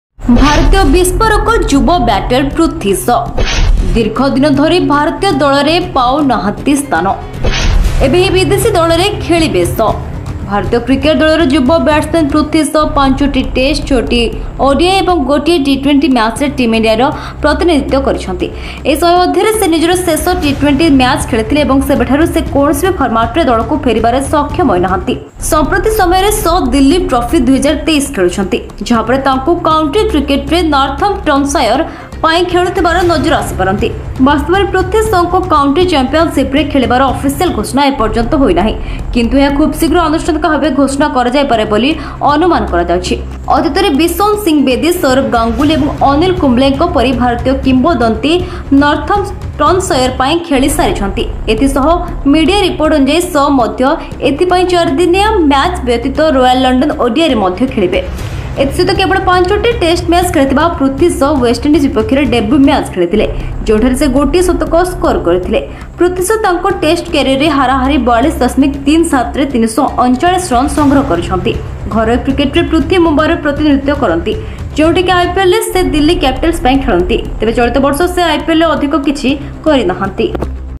Prithvi shaw News| Prithvi Shaw will be seen playing county cricket, will play for this team
Prithvi Shaw will be seen playing county cricket, will play for this team| କାଉଁଟି କ୍ରିକେଟ ଖେଳିବେ ପୃଥ୍ବୀ ଶ.